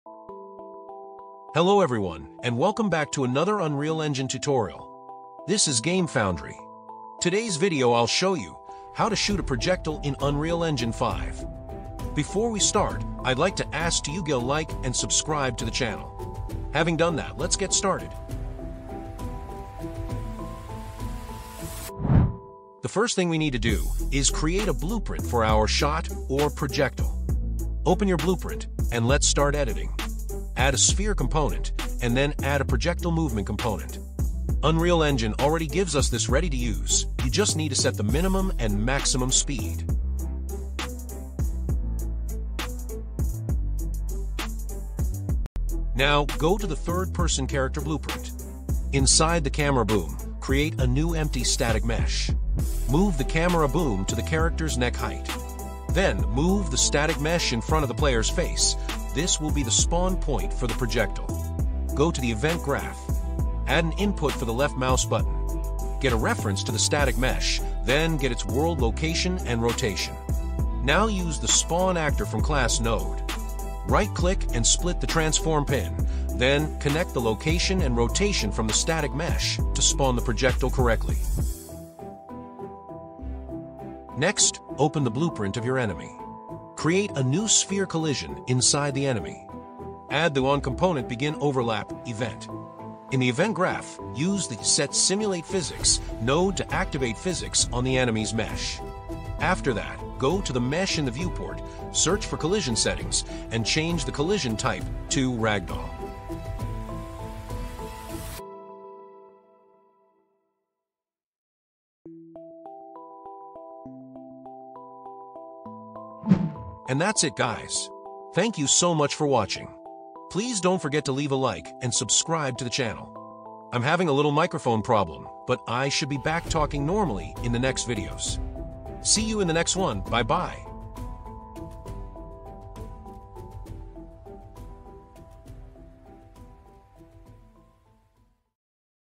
Unreal Engine 5: How to Shoot and Ragdoll Enemies!
Learn how to create a simple but powerful ragdoll system in this step-by-step Unreal Engine 5 tutorial! In this video, I will show you how to shoot a projectile in Unreal Engine 5 that triggers ragdoll physics on your enemies. This method is perfect for beginners or anyone looking to improve their gameplay mechanics with fun and responsive physics.

We will go over how to create a projectile, how to shoot that projectile from your player, and how to set up your enemy so that it reacts with a ragdoll effect when hit. This technique works perfectly in both Unreal Engine 5 and Unreal Engine 4, so no matter which version you're using, you can follow along.

You will also learn how to create an enemy character, how to simulate physics using triggers, and how to trigger an event in a level using collisions. If you're building a combat system, this is a great base to start adding health and damage systems, and later you can even implement a health bar in Unreal Engine 5.

Whether you're working on a shooter, a third-person game, or just experimenting, this is a great way to practice using Unreal Engine's powerful blueprint system. This guide will help you understand how to handle collisions, how to spawn and shoot simple projectiles, and how to add awesome ragdoll effects to your enemies.

If you’re looking for tutorials on how to make an enemy in Unreal Engine 5, or if you just want to see your enemies fly around with physics, this video is for you.